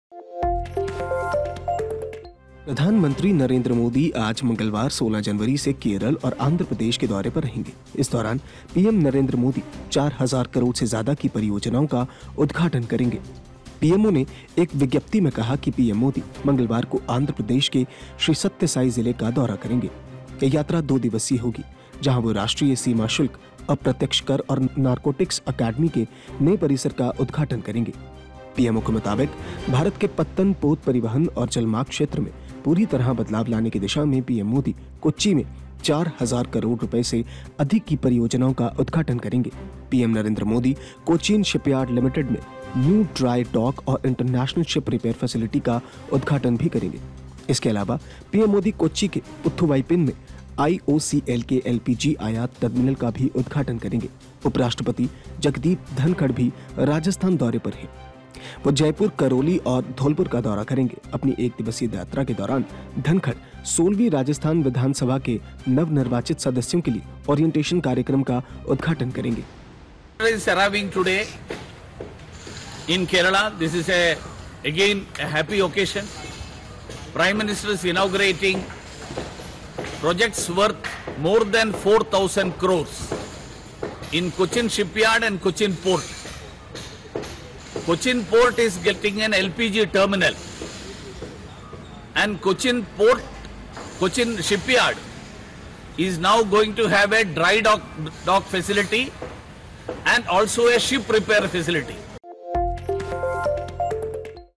PM Modi Visit: आंध्र प्रदेश और केरल के दो दिवसीय दौरे पर आज जाएंगे पीएम मोदी
PM Modi Visit: प्रधानमंत्री नरेंद्र मोदी आज मंगलवार 16 जनवरी से केरल और आंध्र प्रदेश के दौरे पर रहेंगे. इस दौरान पीएम नरेंद्र मोदी 4,000 करोड़ से ज्यादा की परियोजनाओं का उद्घाटन करेंगे.